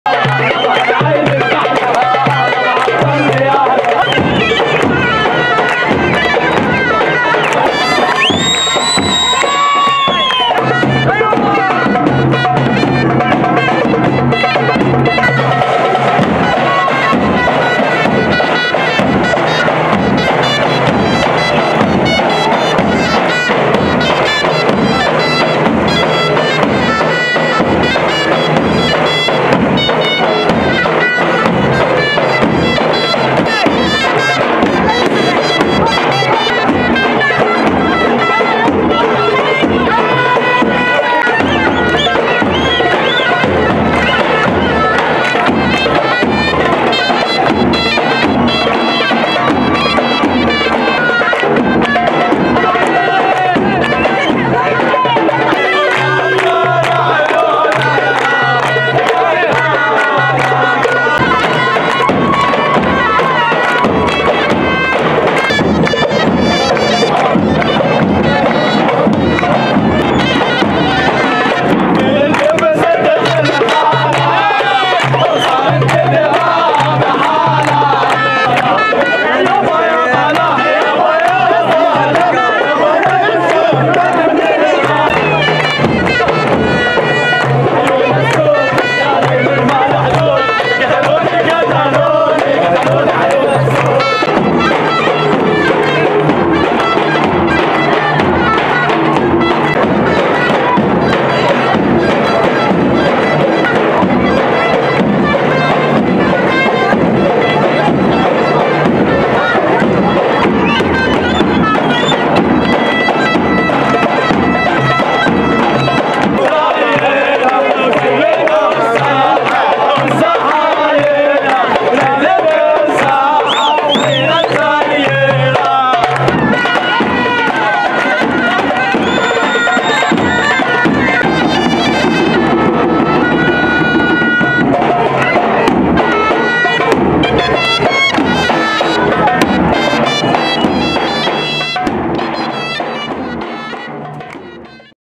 بعرسٍ قرويّ تراثي، زُفَّ العروسان "عدنان و غدير" في ساحة بلدتهما "الشرقية"، على وقع الطبل و المزمار
Nabatieh.TV